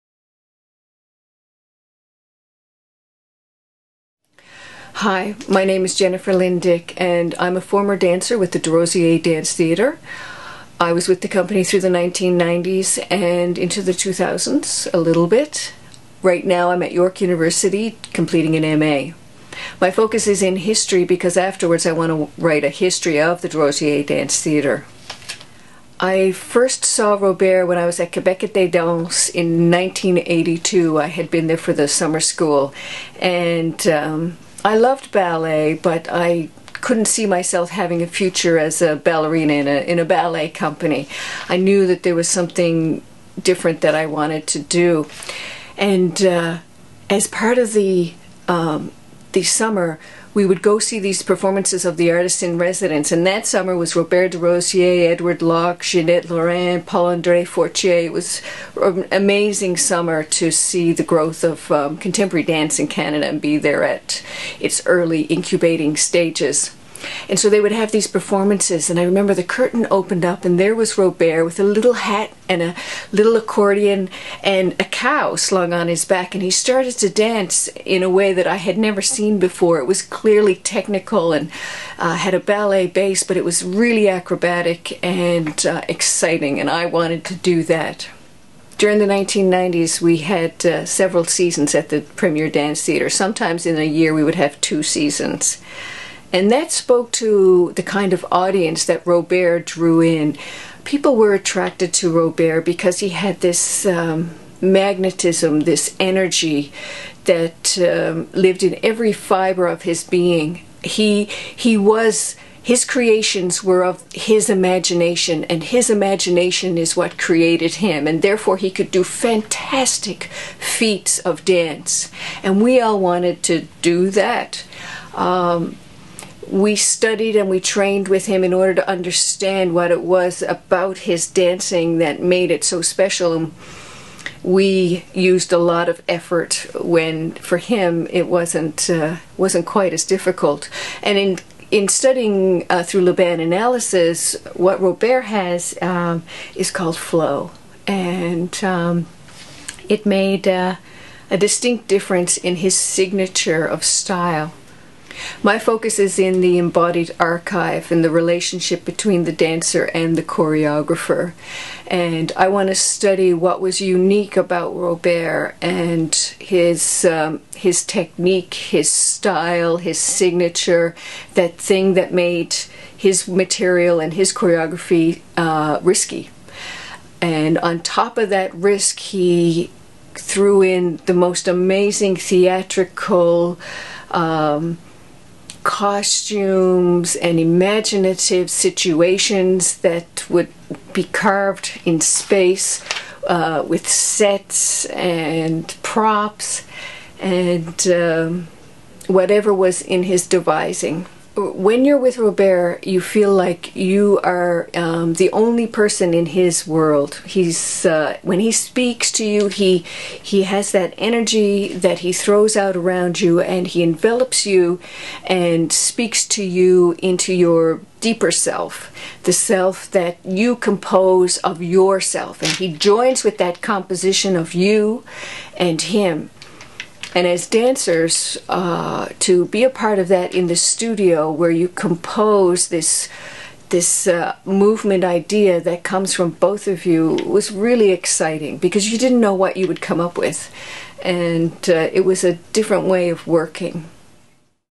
Robert's Influence on My Work
There is only one Robert Desrosiers. Dancer Jennifer Dick sent us this exclusive video detailing Robert's unique influence on her work, from her own performances to her latest project - a history of the Desrosiers Dance Theatre!